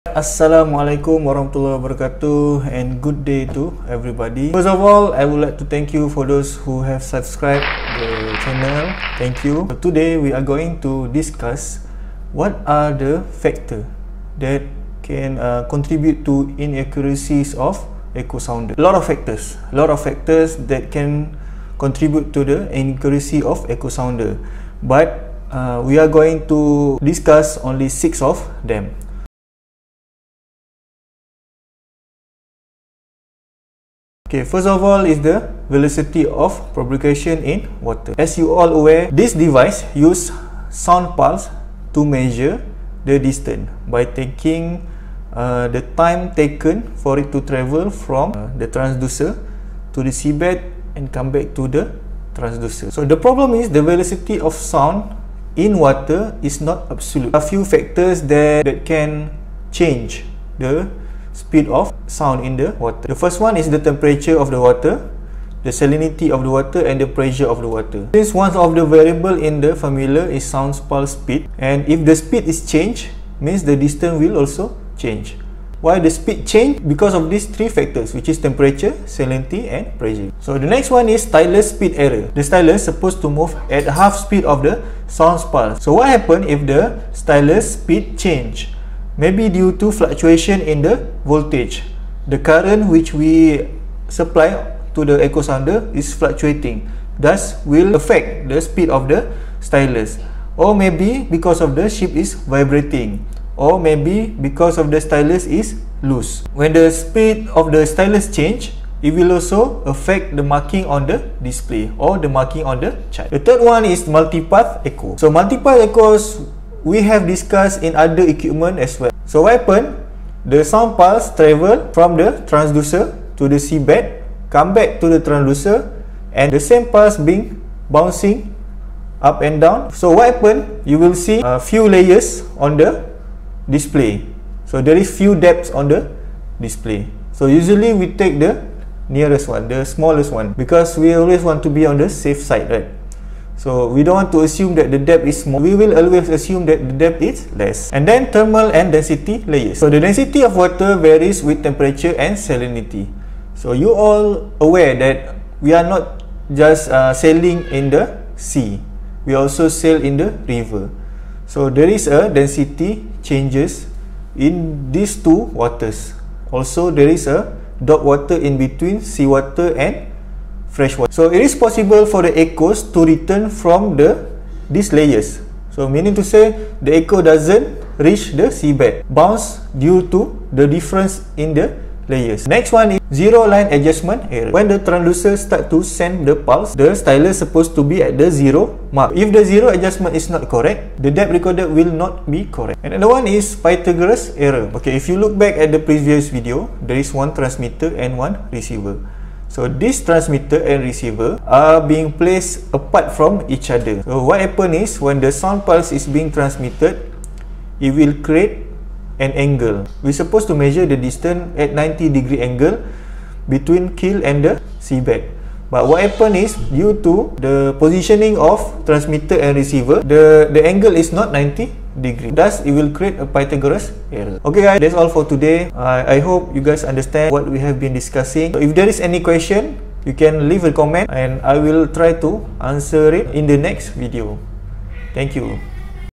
#5 ECHO SOUNDER - INACCURACIES
5th VIDEO ON ECHO SOUNDER . WHAT ARE THE FACTORS THAT CONTRIBUTE TO THE INACCURACIES OF AN ECHO SOUNDER?